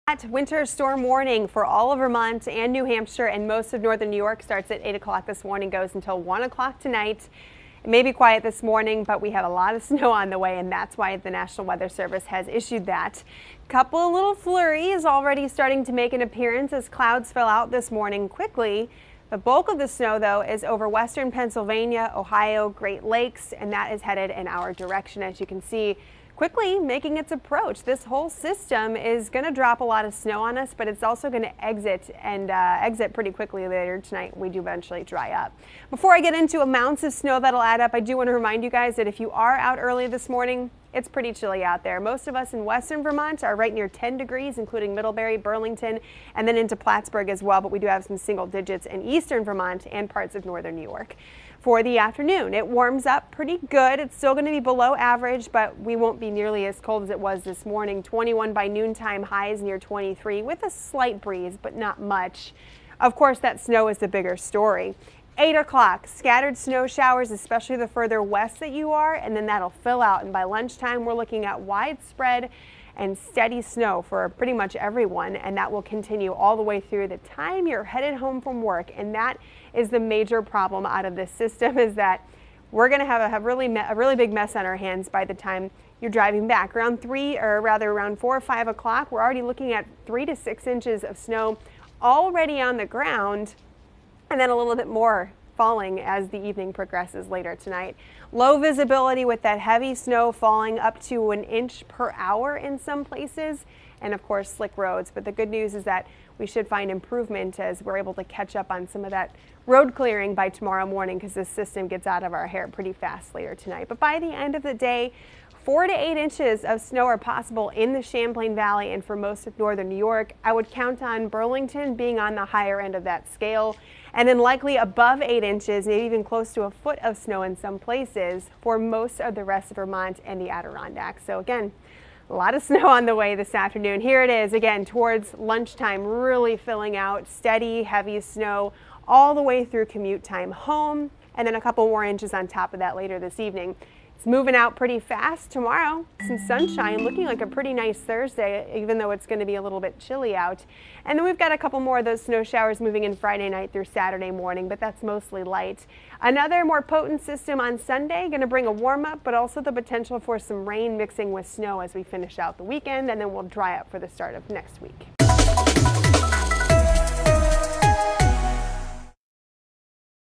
Video: Winter Storm Warning for most of the area today 2/7/18
Steady, heavy snow will fall most of the day